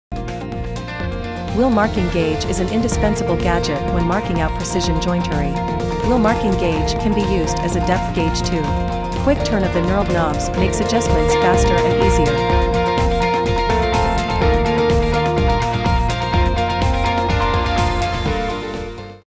120MM Adjustable Wheel Marking Gauge Woodworking Tool Wood Marking Gauge Scribe Tool
1.The hardened steel wheel cutter scribes perfectly at any point of its circumference;Since it cuts wood fibres rather than tears, it marks cleanly, even on cross grain
2.The wheel's single-sided bevel also pulls the gauge face against the stock, maintaining accuracy
3.The cutting edge of the wheel is at the extreme end of the shaft, which is useful for transferring dimensions such as tenon shoulder or mortise depth
This wheel marking gauge is very precise,120mm measuring range,support micro adjustable,the cutter head can retraces into countersink in brass face to protect
4.Excellent materials and well finished,aluminum alloy body, brass face, stainless steel rod; brass face is brushed,gradation marks are laser etched; it's robust and heavy but the body feels good in the hand and the weight balances well